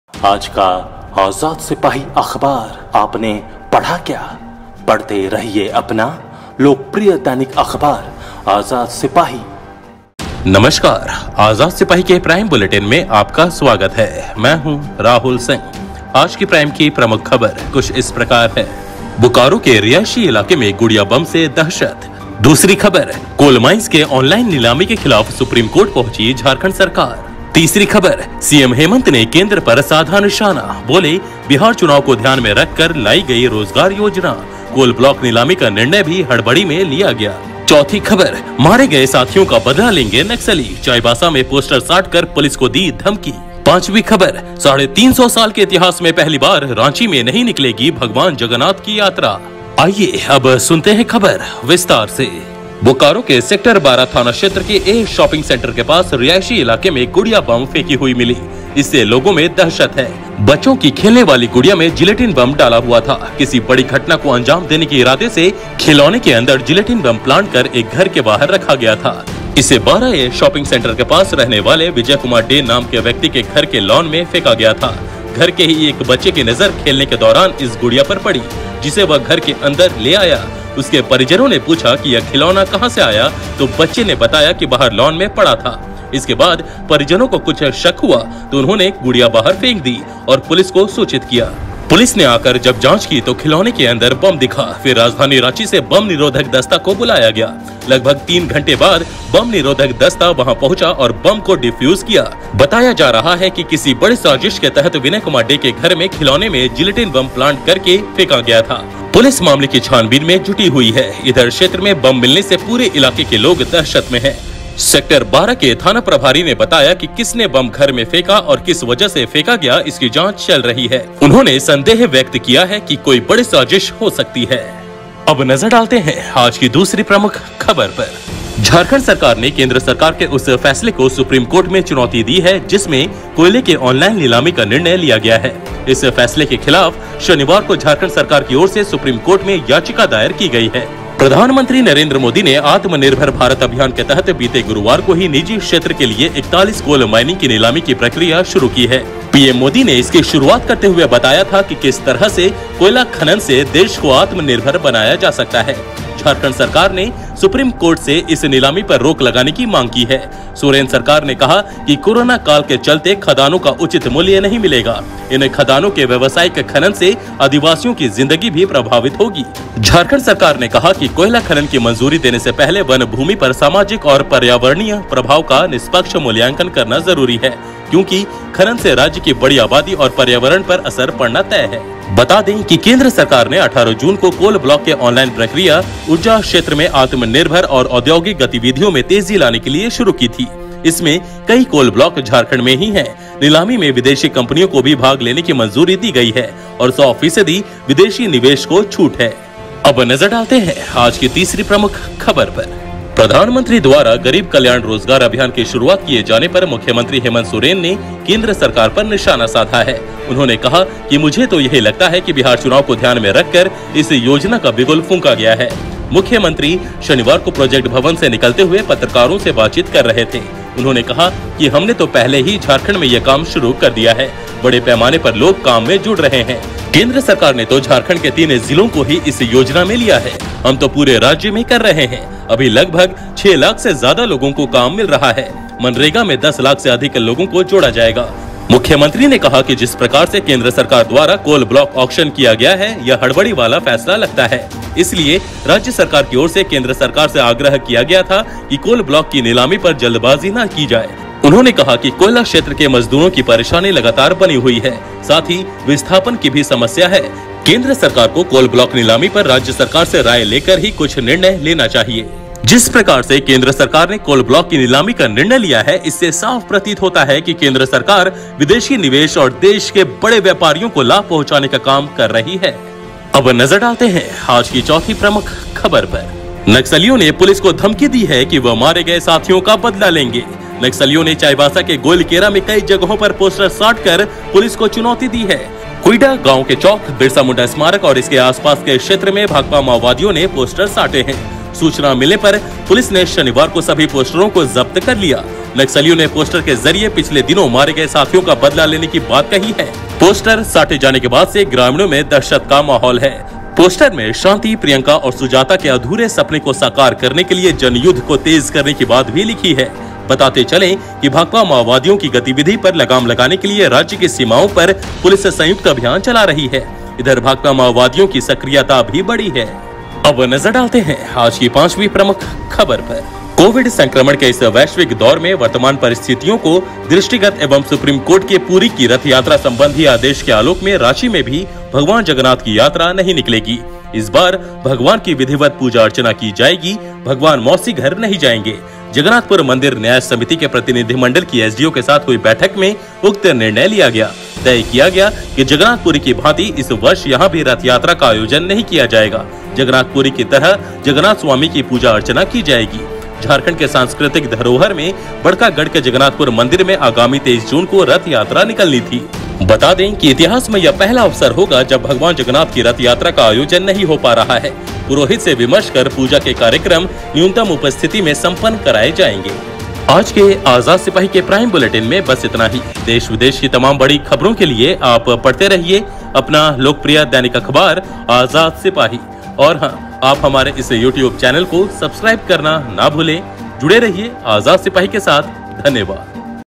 केंद्र के कोयला खदानों की नीलामी के निर्णय के विरोध में सुप्रीम कोर्ट गयी हेमंत सरकार | आजाद सिपाही
1. बोकारो के रिहायशी इलाके में गुड़िया बम से दहशत

2. कोल माइंस के ऑनलाइन नीलामी के खिलाफ सुप्रीम कोर्ट पहुंची झारखंड सरकार

3. सीएम हेमंत ने केंद्र पर साधा निशाना, बोले-बिहार चुनाव को ध्यान में रखकर लायी गयी रोजगार योजना, कोल ब्लॉक नीलामी का निर्णय भी हड़बड़ी में लिया गया

4. मारे गये साथियों का बदला लेंगे नक्सली, चाईबासा में पोस्टर साट कर पुलिस को दी धमकी

5. साढ़े तीन सौ साल के इतिहास में पहली बार रांची में नहीं निकलेगी भगवान जगन्नाथ की यात्रा